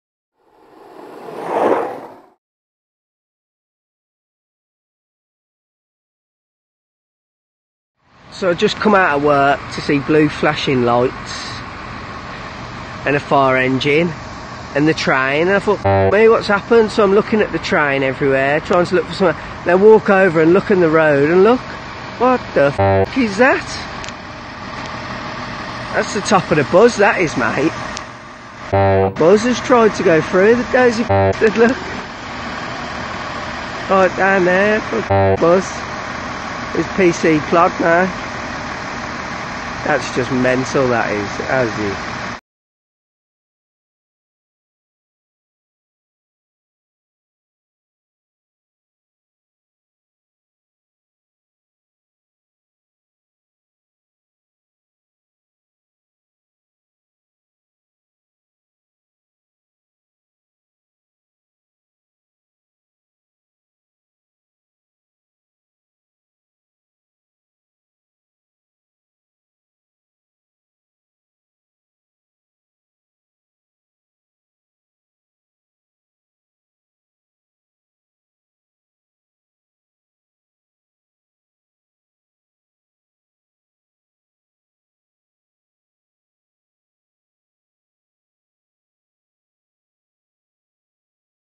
Bus V Bridge
A double decker bus miss judged the height of a bridge on Bournville Lane, Bournville. Fortunately, the bus wasn't in service at the time and nobody was injured